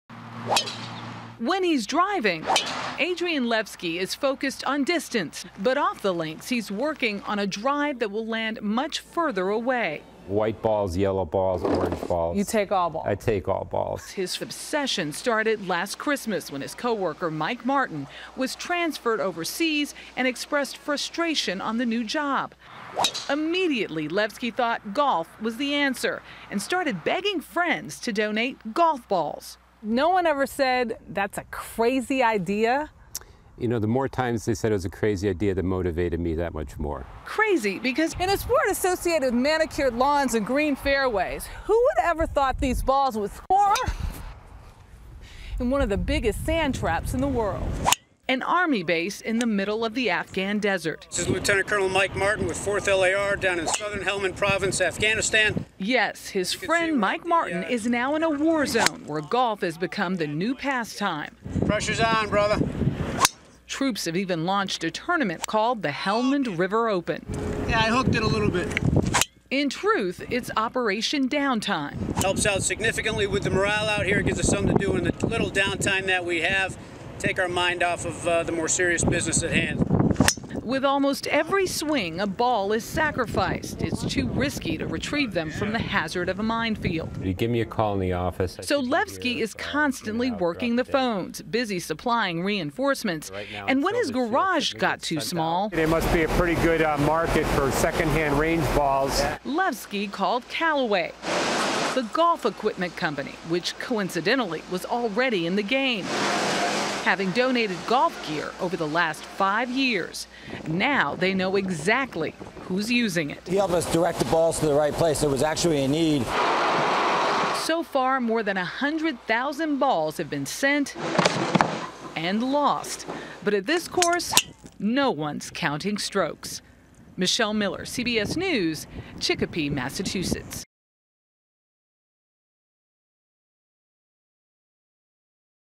War-Zone Golf: New Past-Time?
An investment banker's idea to send donated golf balls overseas became a hit for troops at an Army base. As Michelle Miller reports, golf has become a new past-time in this war zone.